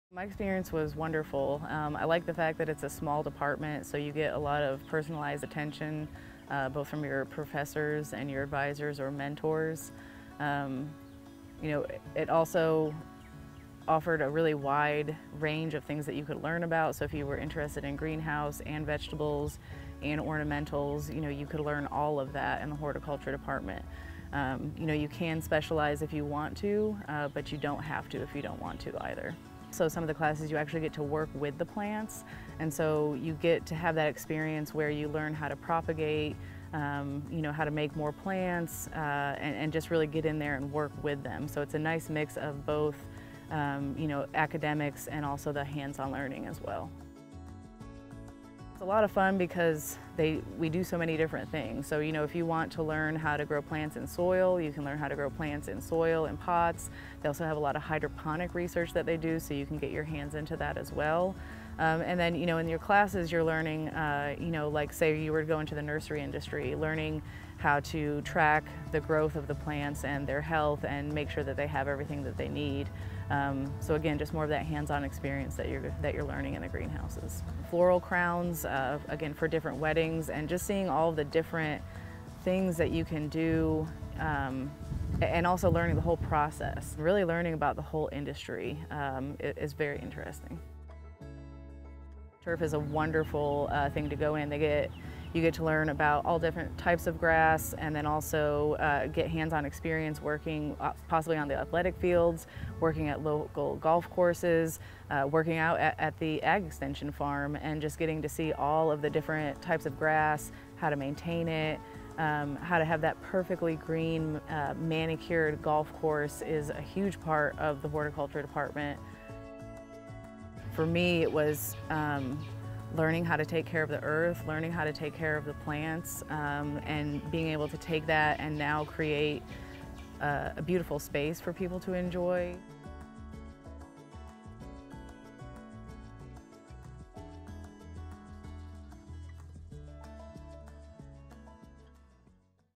What is Horticulture, Landscape and Turf Science?
What exactly is horticulture, landscape and turf science and why should you study it? Learn more about how you can have a dynamic career in sports turf management, research, viticulture, floral production, sales and more with a Horticulture, Landscape and Turf Science degree from the Dale Bumpers College of Agricultural, Food and Life Sciences at the University of Arkansas.